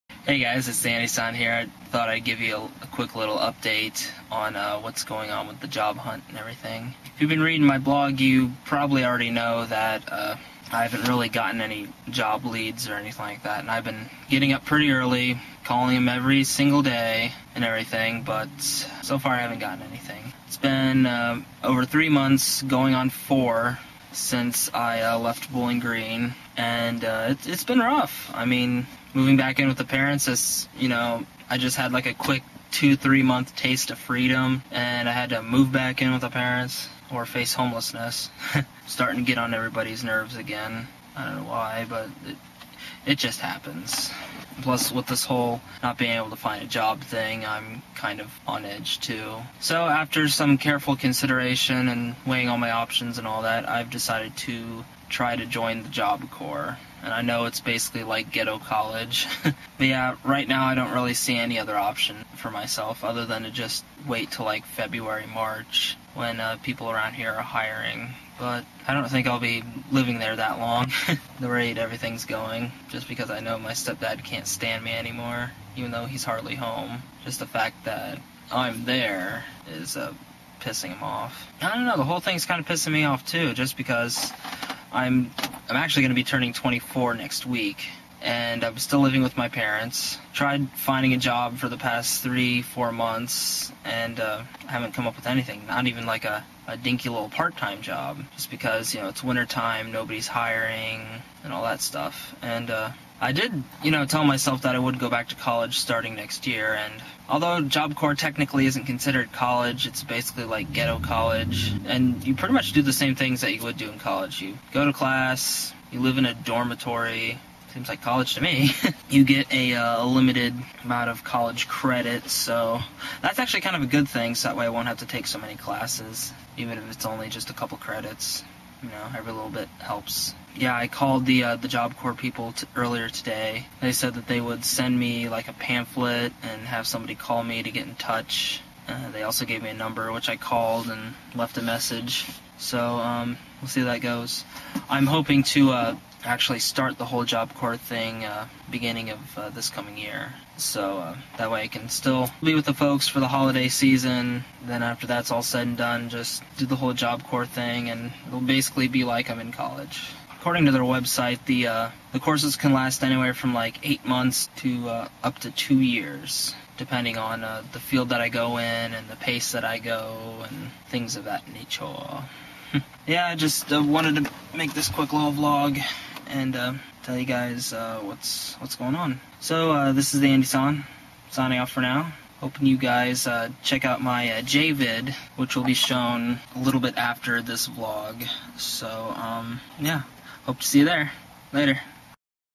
Job Corps 4 Me - Vlog 027 - TheAndySan
I'm just giving you guys a quick update on my job hunt. I've been looking for a job for the past three months and I haven't gotten anywhere.

Because I doubt that my stepdad will let me stay rent-free for any longer, I've decided to go into the Job Corps. From what I understand, it's basically like college in that you dorm there and take classes there as well.

If any of you guys are in the Job Corps or have gone there, please leave me a comment telling me about your experiences there and if you'd recommend it.

TheAndySan